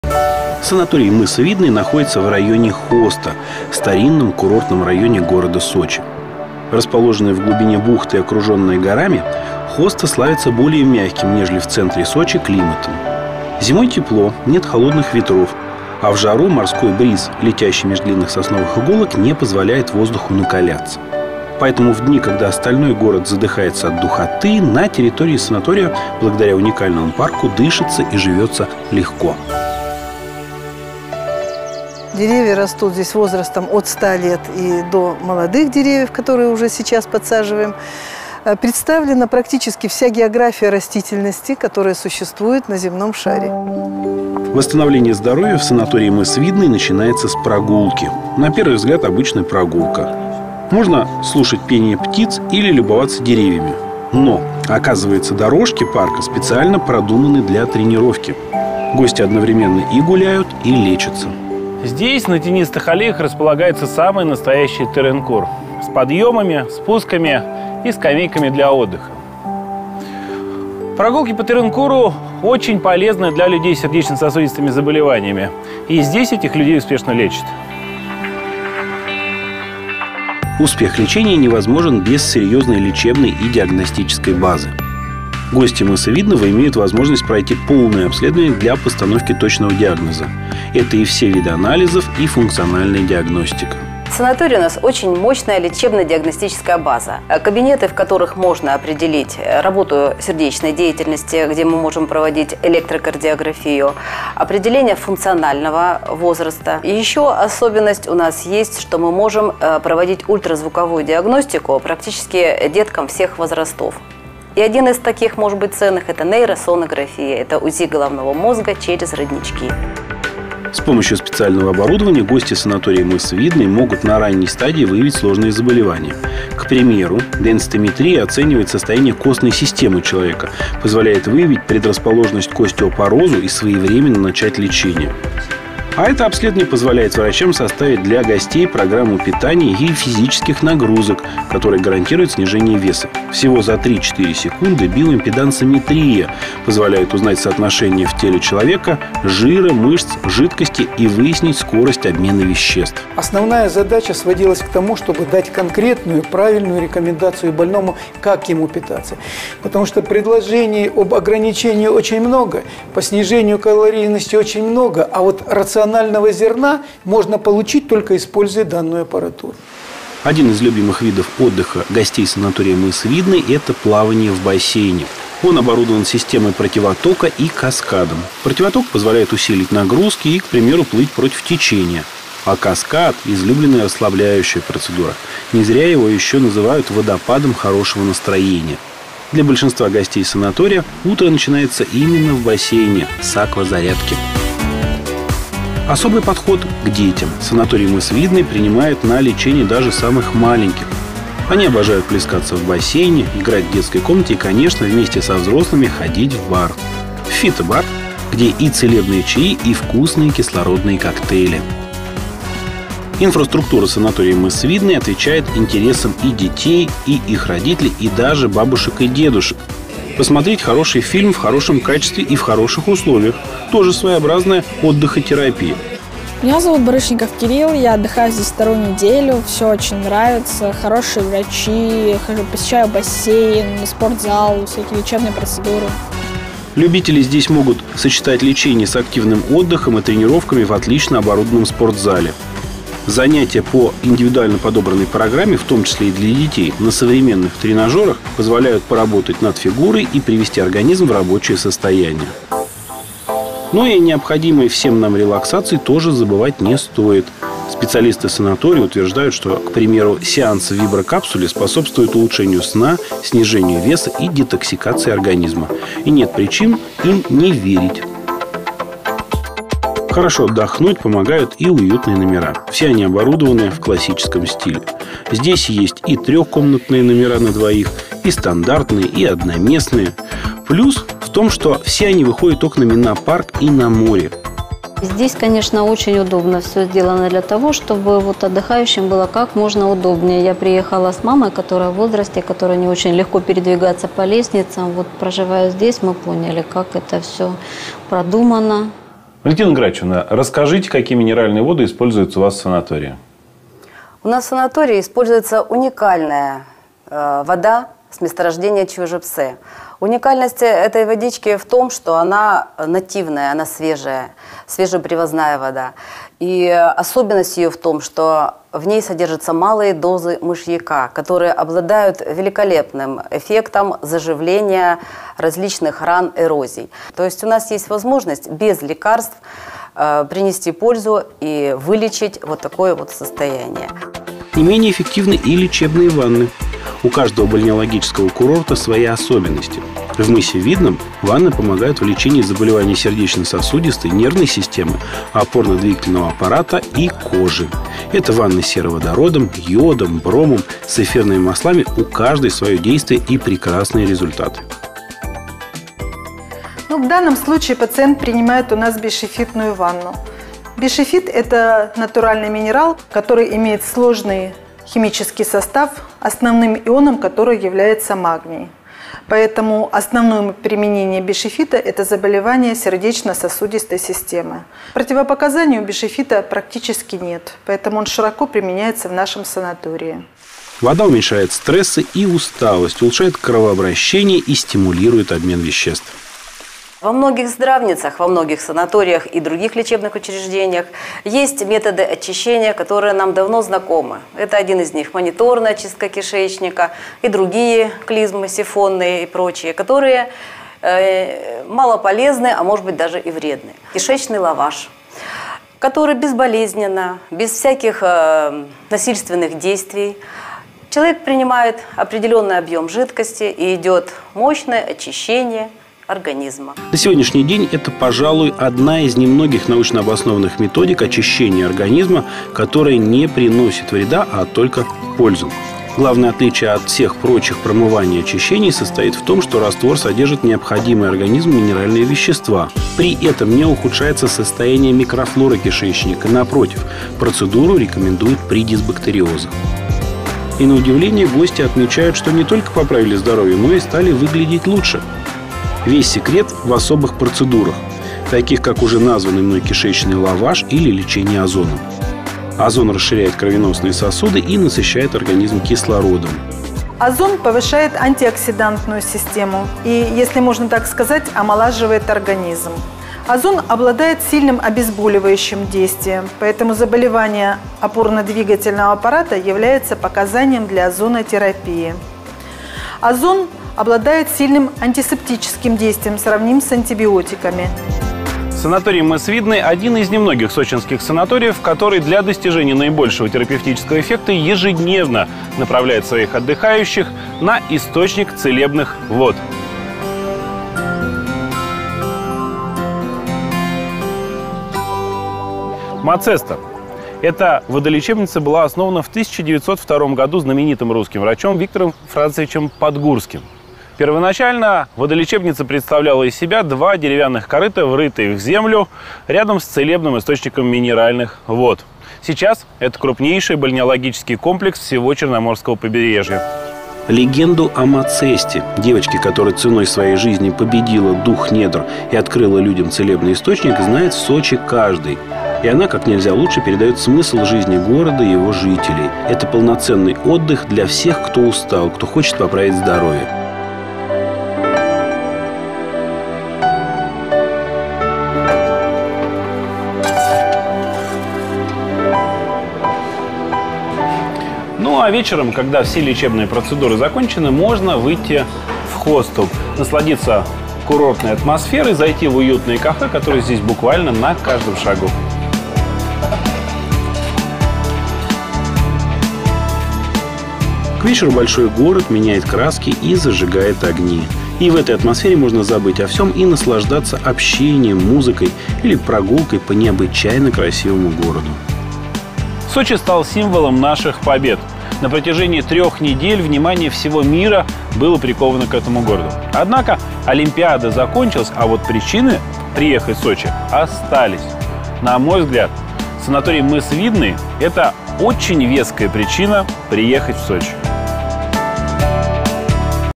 Санаторий "Мыс Видный" г.Сочи
Мыс Видный - это лучшее место на побережье города Сочи. Чистый воздух и естественная тишина поселка Хоста позволят Вам получить редкое удовольствие, если Вы посетите наш санаторий. Персонал санатория состоит из высококвалифицированных и сертифицированных сотрудников, полностью отвечающих за Ваше здоровье и хорошее настроение в период отдыха.

Отдел бронирования: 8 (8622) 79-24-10, сот. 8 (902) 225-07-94